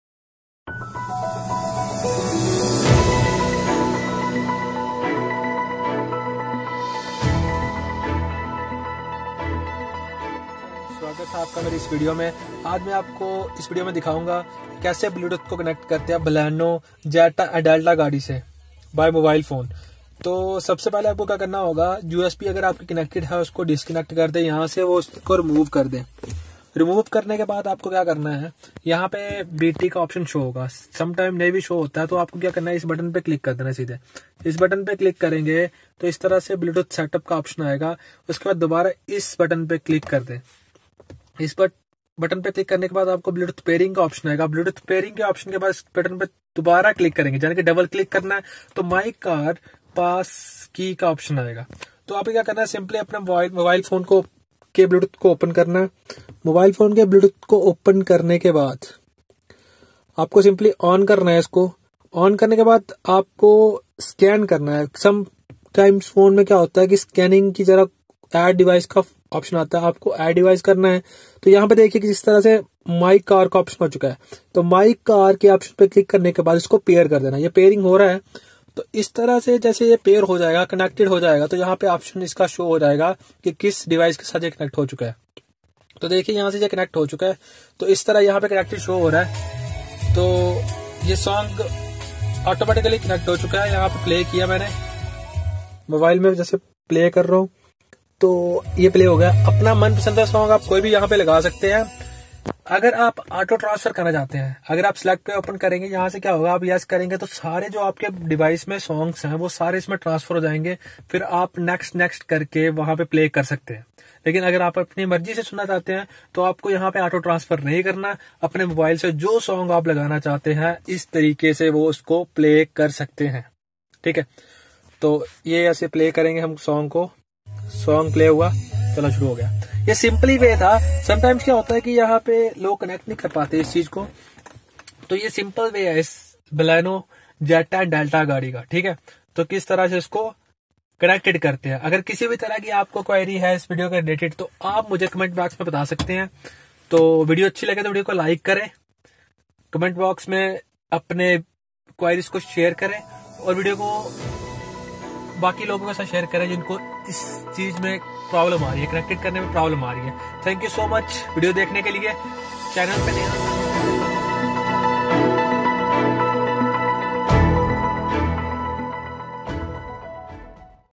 how to Connect Mobile Bluetooth with Baleno Car | Connecting Bluetooth speaker | Baleno Zeta/Delta
In this video, I show you how to connect mobile bluetooth with baleno car

Video by Ajay k. Bhagat Also Connected with me